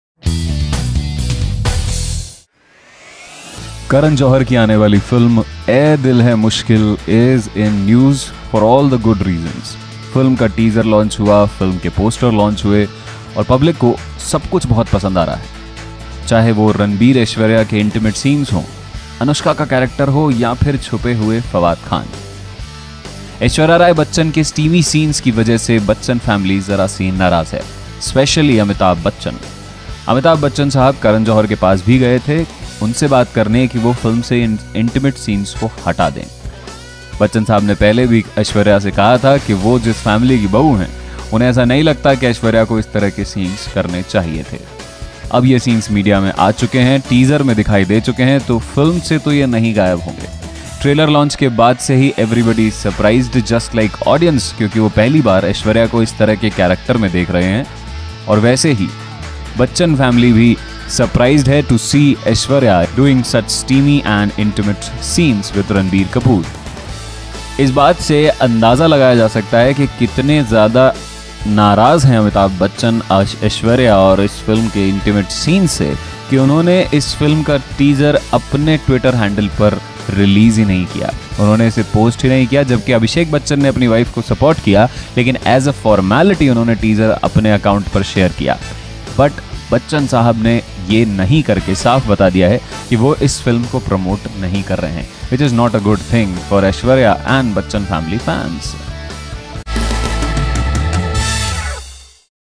Amitabh Fuming Over Aishwarya Steaming Scenes | Ae Dil Hai Mushkil | New Bollywood Movies News 2016
The first teaser trailer of Karan Johar’s upcoming romantic flick ‘Ae Dil Hai Mushkil’ has been making all the right noises around town. Everything, from its intriguing story line to Ranbir Kapoor and Aishwarya Rai Bachchan’s steamy scenes along with Fawad Khan’s appearance and Anushka Sharma’s character has kept fans guessing about the film. However, Aishwarya’s bold scenes with Ranbir ultimately stole the show, as it was the first time that fans got to see the former beauty queen play such a character.

Reports now claim that these scenes have got Ash’s father-in-law, Amitabh Bachchan seeing red. Apparently, the superstar is fuming over Aishwarya steaming up the screen with Ranbir. It had earlier been reported that Big B even approached Karan to cut off her kissing scene in the film. The makers thought the intimate scenes wouldn’t be a problem, as nothing was flagged during the narrations. However, like the audience, even Aishwarya’s family was ‘stunned to see her bold avatar’.

There are also reports that state Amitabh, Abhishek and Aishwarya got into a ‘tiff’ on the day of the trailer launch, as Bachchan Sr. was fuming over the steamy scenes. Abhishek put up a very supportive front and according to reports, shared the teaser ‘as a formality’. Amitabh, claim the reports, however, stuck to his guns and refused to budge from his decision of not sharing the link on his social networking handles.

However, considering the Bachchan family are thorough professionals, we would take these rumours and reports with a pinch of salt.